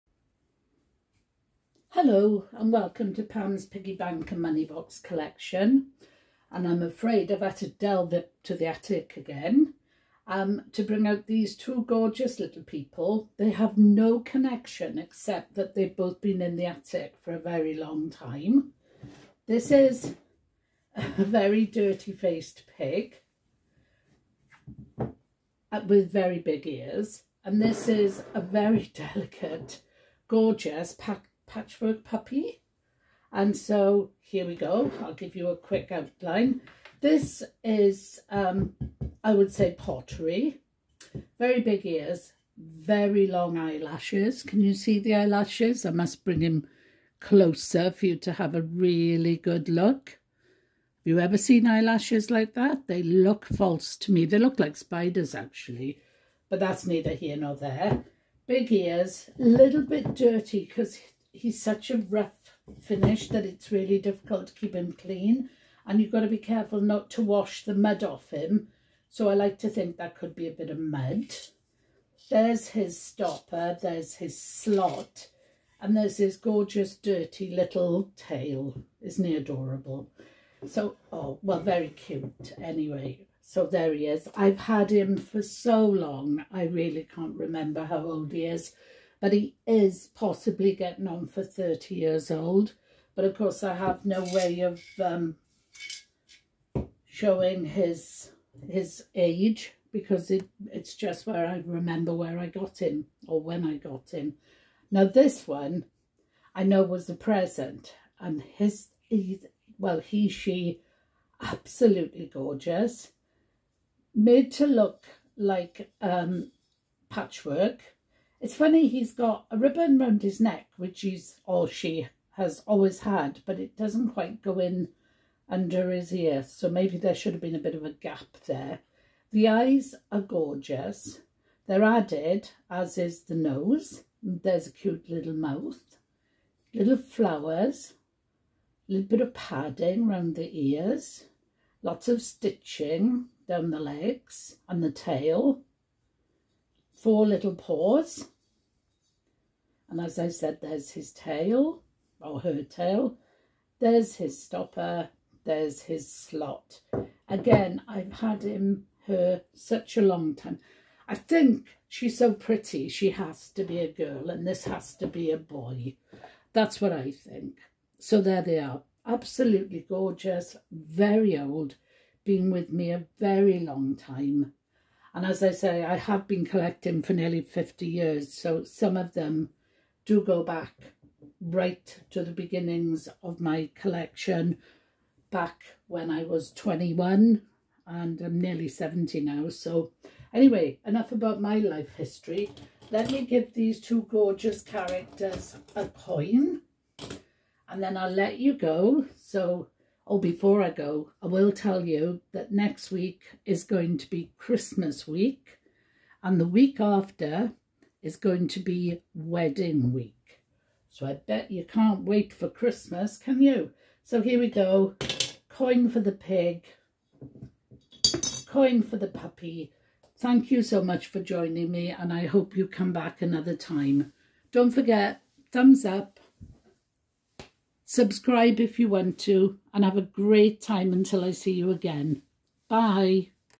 pig and the puppy
Here we have to lovely moneyboxes. A pig with a very dirty face. Wonder what he's been up to. A very cute well to do puppy